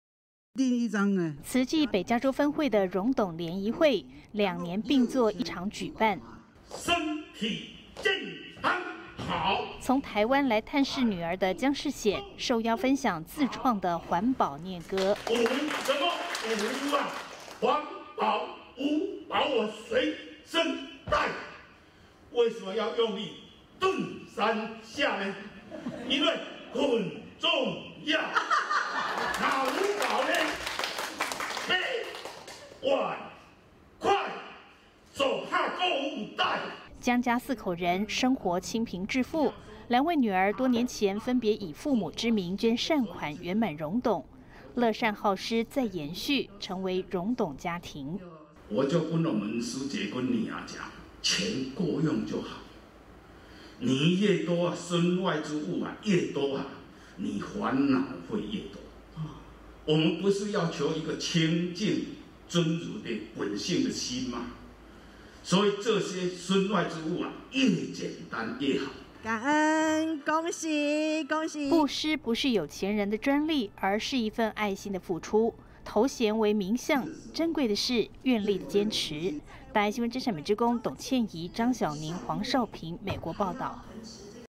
以環保為志業 退而不休唱作俱佳
慈濟志工江世顯，今年77歲，他從環保署退休後，就投入環保。今年，和太太到美國探視兩位女兒，在榮董聯誼會，分享自創的環保唸歌。

慈濟北加州分會的榮董聯誼會，兩年併作一場舉辦。

從台灣來探視女兒的江世顯，受邀分享自創的環保唸歌。

慈濟志工 江世顯：「五什麼五啊 環保五寶 我隨身帶為什麼要用力頓三下呢，因為很重要 哪五寶呢，杯 碗 筷 手帕 購物袋。」

江家四口人，生活清平致富，兩位女兒多年前分別以父母之名捐善款、圓滿榮董；樂善好施 再延續，成為榮董家庭。

慈濟志工 江世顯：「我就跟我師姊和女兒講錢夠用就好 你越多身外之物，越多啊 煩惱會越多，我們不是要求一個清淨，真如的本性的心嗎，所以這些身外之物 越簡單越好。」

布施不是有錢人的專利，而是一分愛心的付出；頭銜為名相，珍貴的是，願力的堅持。

真善美志工 董倩宜 張曉寧 黃少平 美國報導